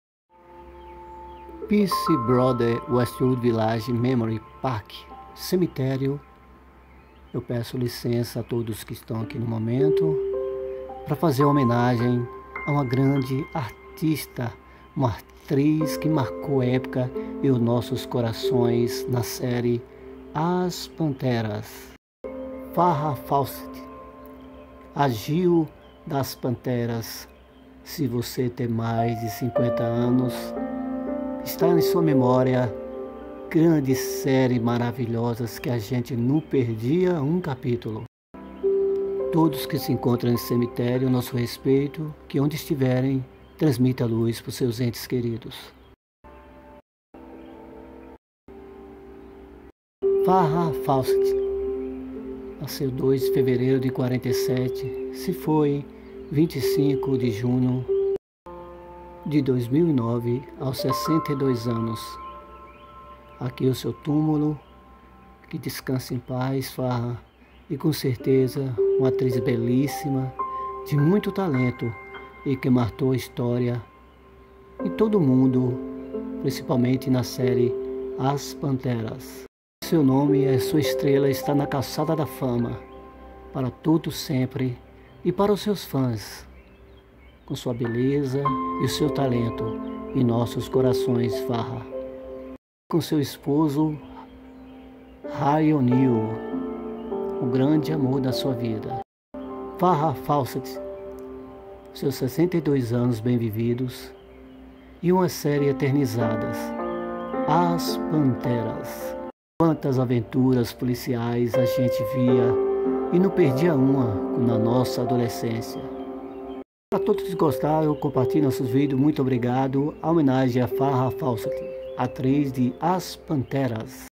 A MORADA O TÚMULO FARRAH FAWCETT ATRIZ DA SÉRIE AS PANTERAS SEU PERSONAGEM ERA JILL MUNROE
O TÚMULO FARRAH FAWCETT ATRIZ DA SÉRIE AS PANTERAS A JILL MUNROE PIERCE BROTHERS WESTWOOD VILAGEM CALIFÓRNIA

Farrah Leni Fawcett (Corpus Christi, 2 de fevereiro de 1947 - Santa Mônica, 25 de junho de 2009) foi uma atriz e modelo estadunidense. É considerada um dos maiores símbolos femininos da década de 1970.

 Várias vezes indicada aos prêmios Emmy e Golden Globe, ganhou fama internacional ao interpretar a detetive particular Jill Munroe na série de televisão Charlie's Angels (As Panteras) .

Mais tarde, ganhou a aprovação da crítica em peças off-Broadway e vários papéis em telefilmes e minisséries (The Burning Bed, Nazi Hunter: The Beate Klarsfeld Story), Poor Little Rich Girl: The Barbara Hutton Story, Margaret Bourke-White e até em papéis antipáticos (Small Sacrifices).

Também foi um ícone da cultura pop, cujo penteado foi copiado por milhões de mulheres e cujo pôster quebrou recordes de venda, tornando-a uma das mulheres mais lindas do mundo dos anos 1970 e 1980, com maior impacto sobre a geração adolescente da época e até depois.